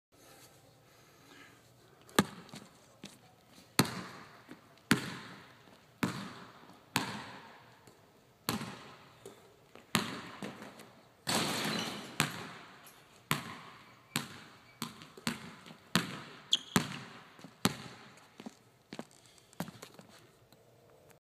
Tobias Borg
Efter två månader på skadelistan är Tobias Borg tillbaka på planen. För att fira detta bjuder den 183cm korta pojkspolingen på en härlig bakåtdunk.